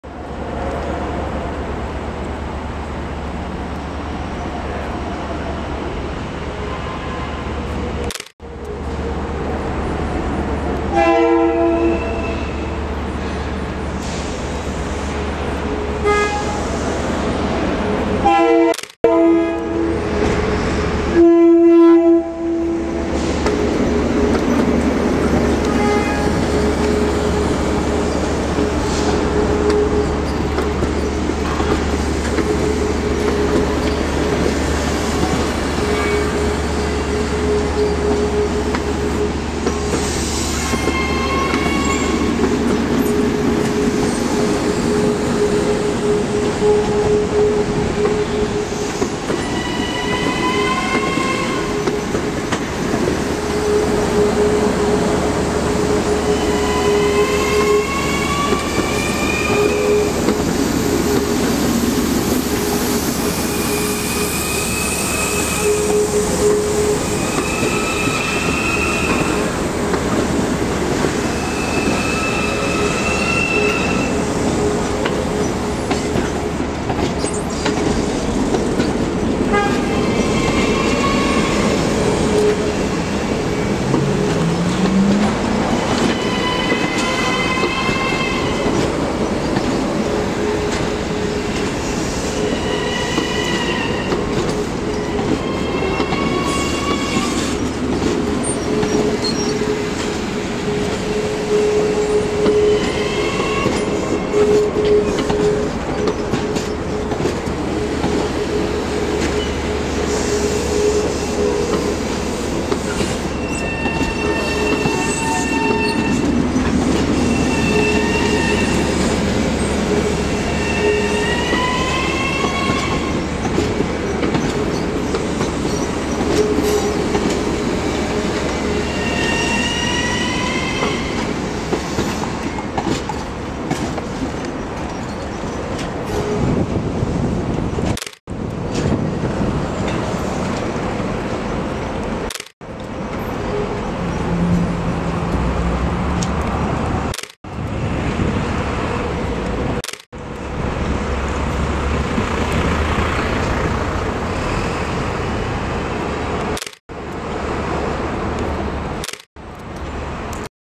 SPECIAL: LIRR M7 Entering Long Island City Yard and Station.
This ia a special video taken in Late September of 2010. Here we see a LIRR M7 Entering Long Island City Yard and Station at a Grade Crossing view in Long Island City, NY.